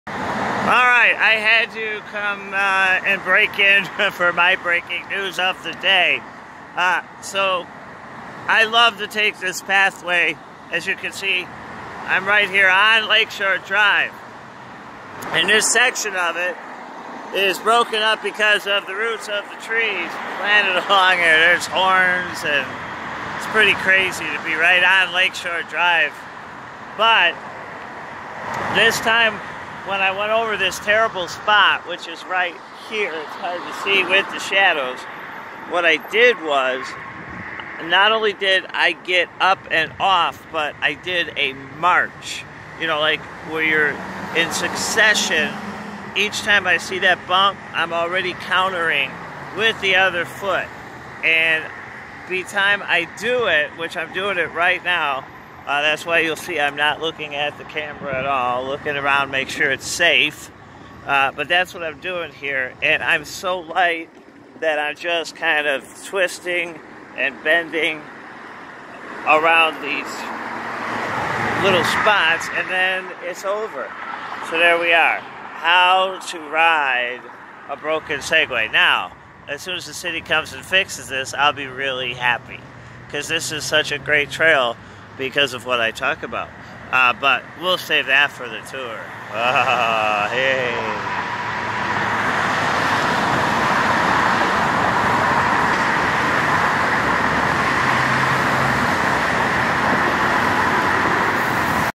Learnin how to ride bumpy sidewalks
6/28/21 roots of trees really destroyed sidewalks in Grant Park Chicago.